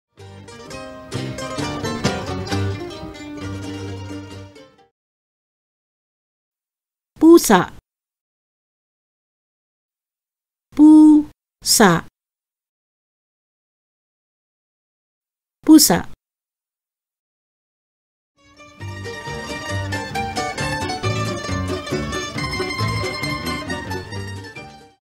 How to pronounce PUSA - Tagalog Plus
How to pronounce PUSA (cat) in Tagalog (Filipino language)? Listen and practice.

Build your Tagalog vocabulary and improve your pronunciation through our learning videos.

Published by TAGALOG PLUS on August 21, 2020.